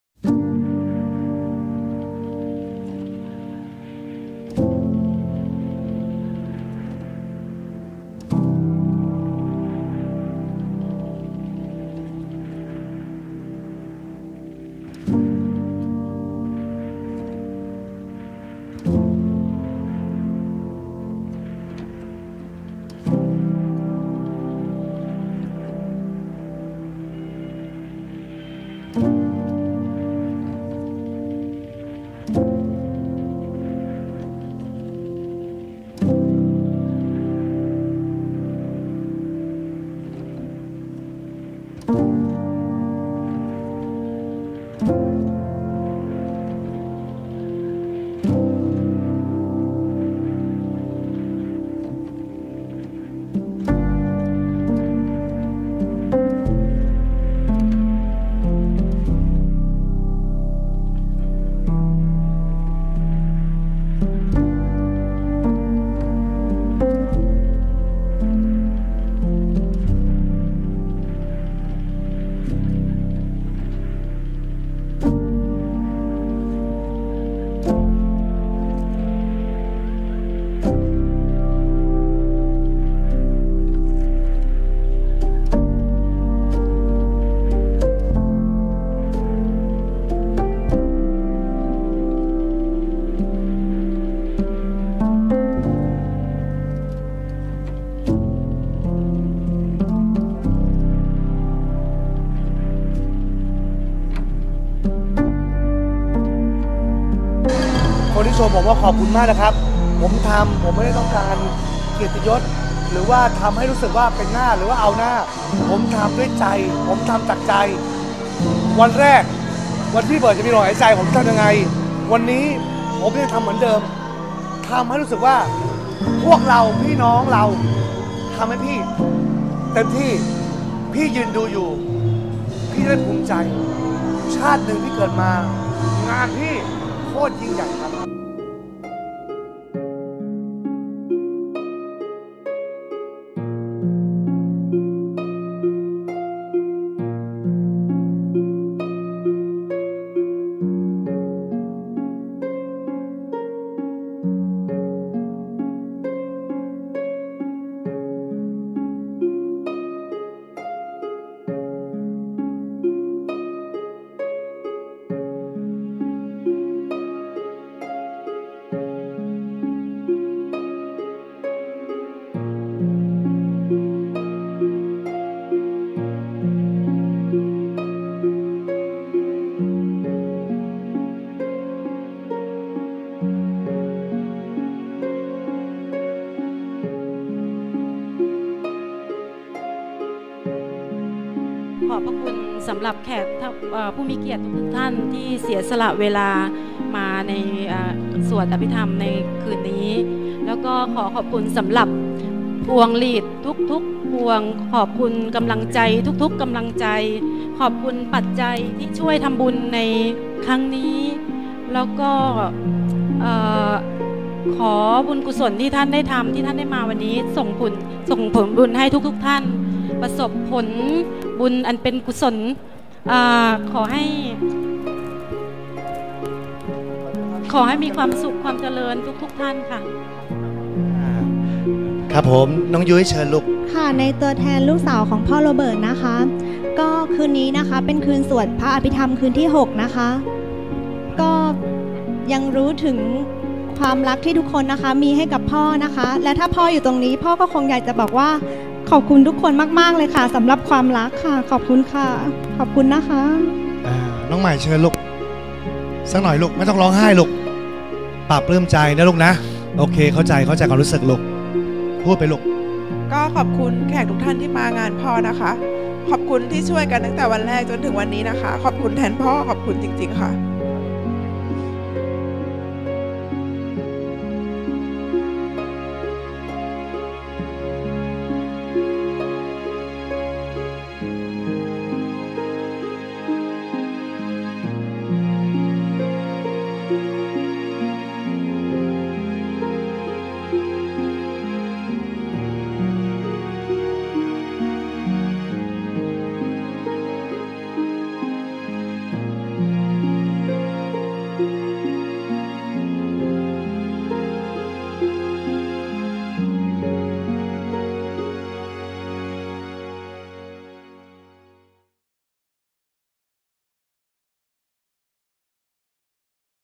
บรรยากาศค่ำคืนที่5 รัก..อาลัย
พิธีสวดพระอภิธรรม นายไพฑูรย์ พุ่มรัตน์ ศาลา1